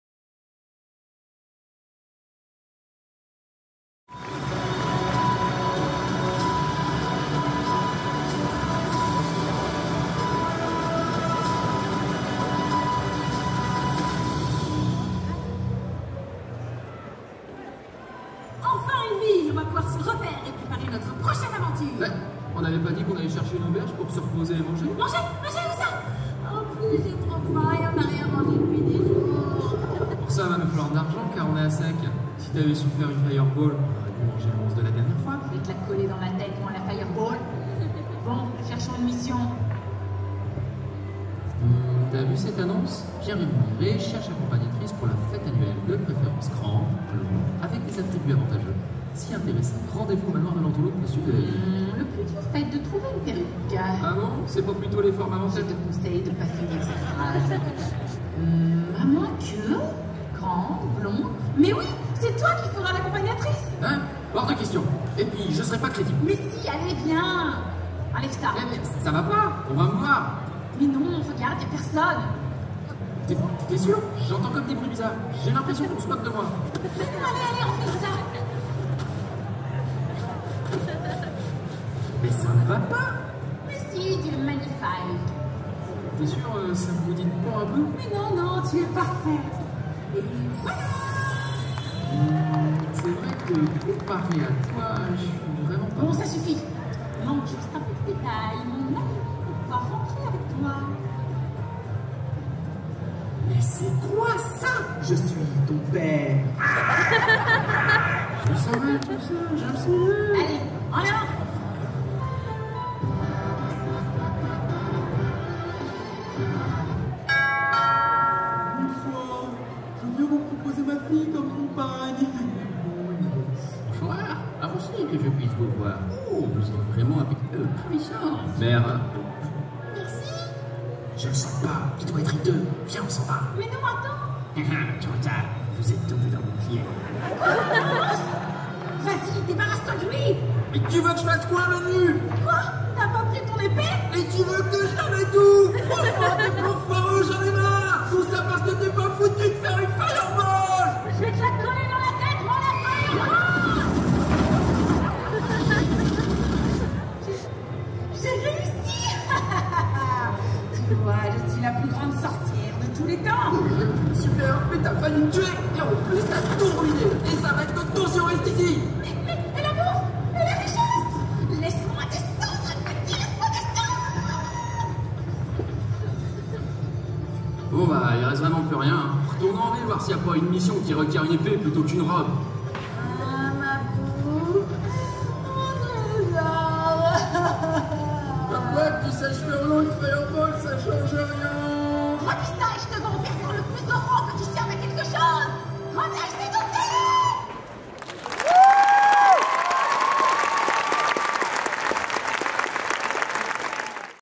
LGS 2019 - Cosplay contest - Nad Cosplay (Slayers - Lina Inverse & Gourry Gabriev)
Lyon Game Show (LGS) 2019  (Lyon // France), CICAF and CFC French Preliminaries, Solo's and Groups!

Slayers - Lina Inverse & Gourry Gabriev

Gectou4